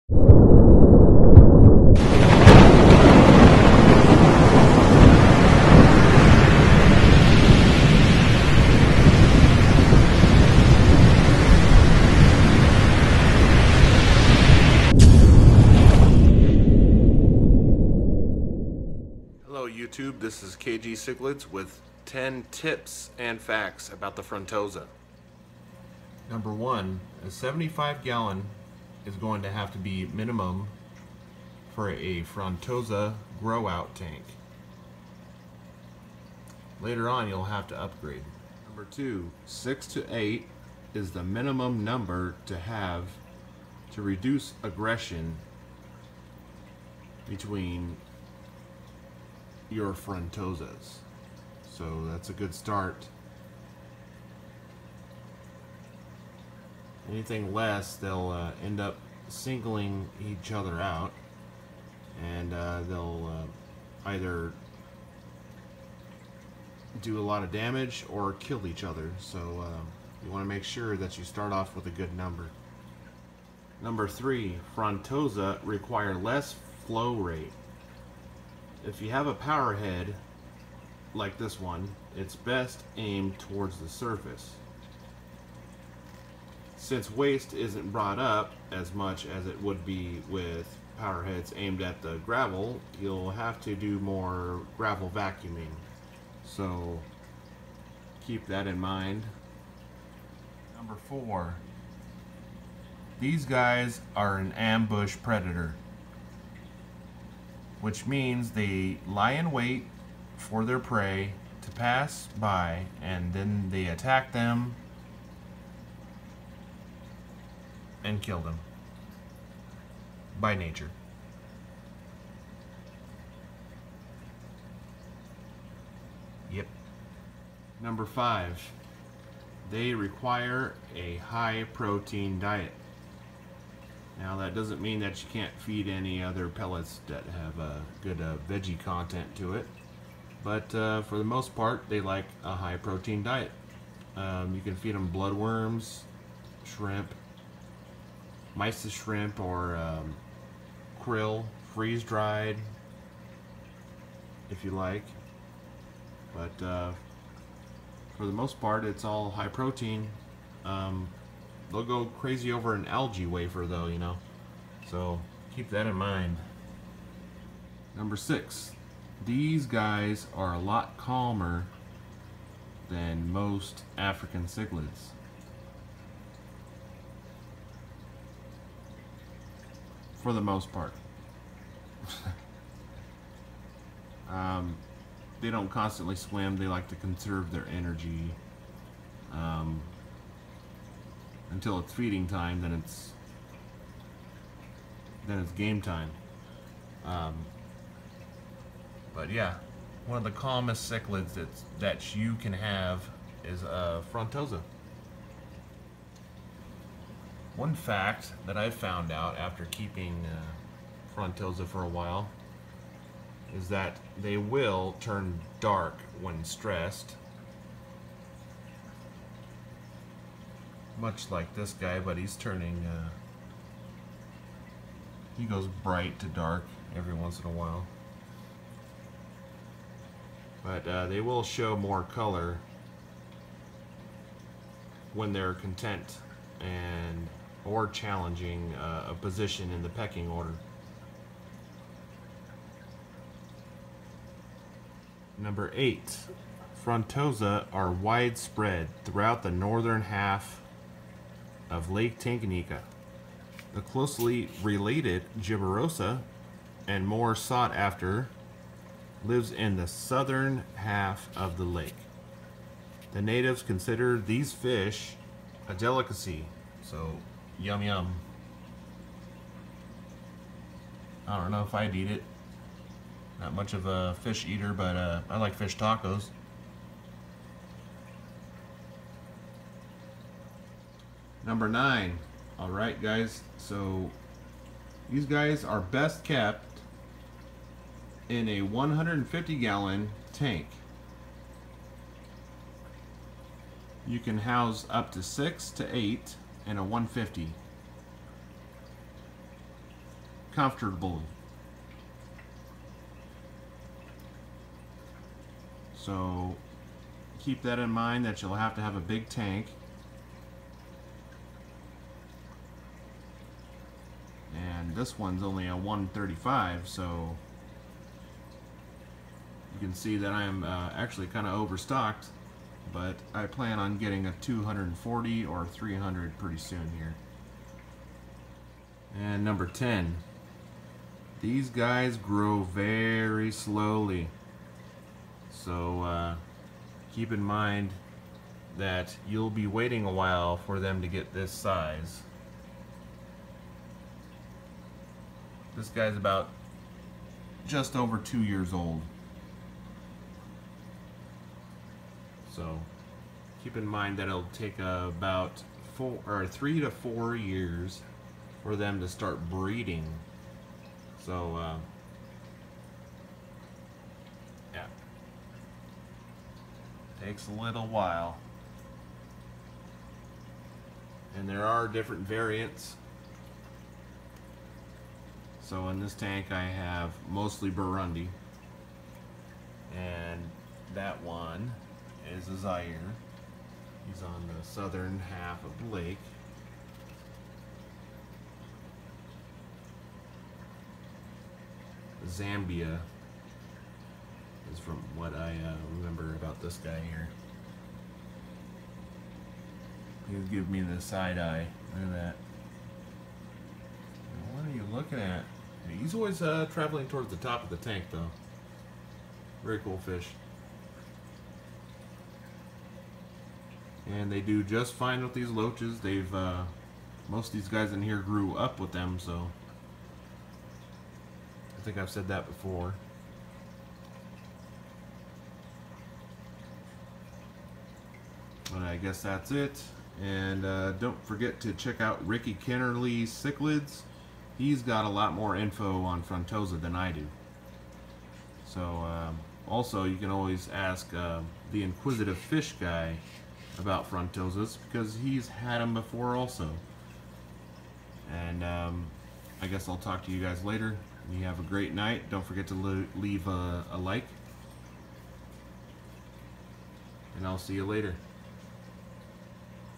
10 facts and tips about frontosa
Be sure to check out Ricky kenerly Cichlids and IFG the inquisitive fish guy